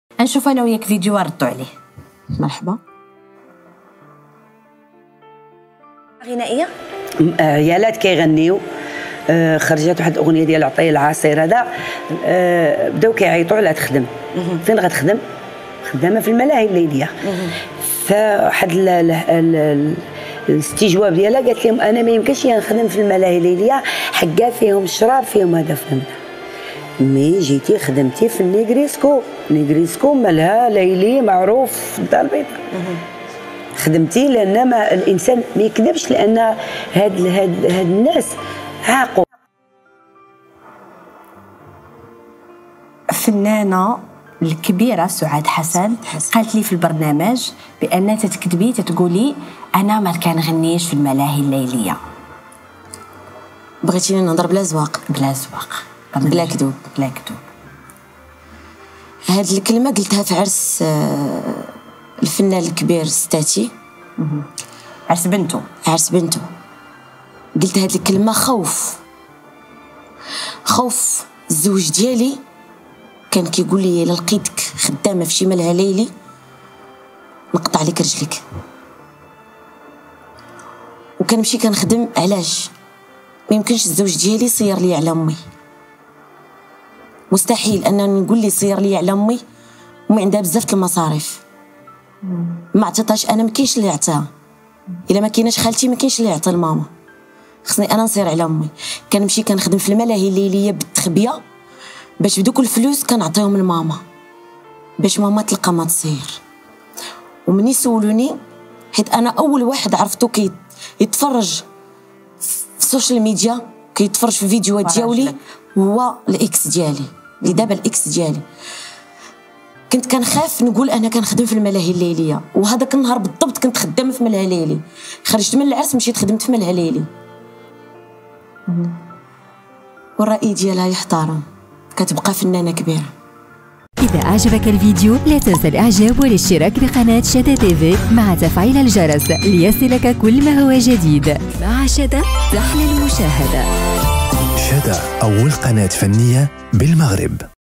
أميمة باعزية توضح : هذا هو السبب لي خلاني نقول أنني ماتنغنيش في الملاهي الليلية
CHADA TV | أميمة باعزية توضح : هذا هو السبب لي خلاني نقول أنني ماتنغنيش في الملاهي الليلية

1ère Chaîne Artistique au Maroc